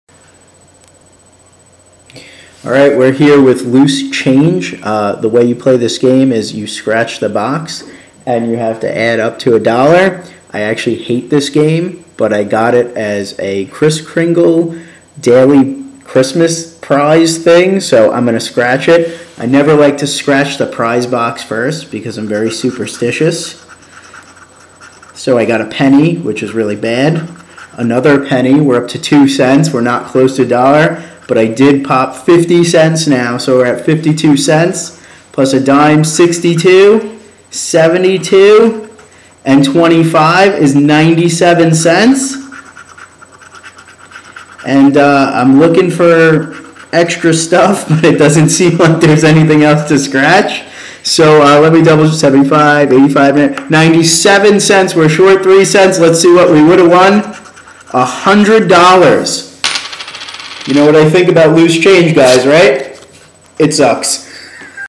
MrScratchLotto bring you Loose Change a New York Scratch Lotto Game
MrScratchLotto tries to win the big bucks by playing the scratch lotto game Loose Change.